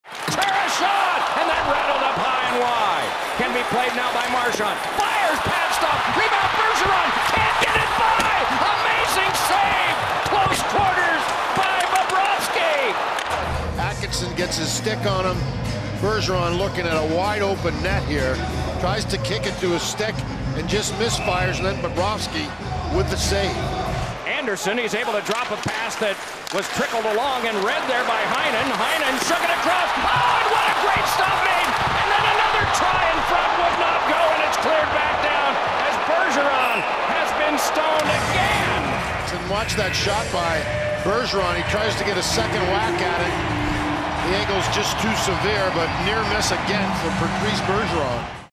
Sergei Bobrovsky slams the door on Patrice Bergeron in OT
Sergei Bobrovsky keeps up with a tough rebound attempt from Patrice Bergeron in overtime, and later in the period, denies him on the rush with help from the side of the net!

Feeling social?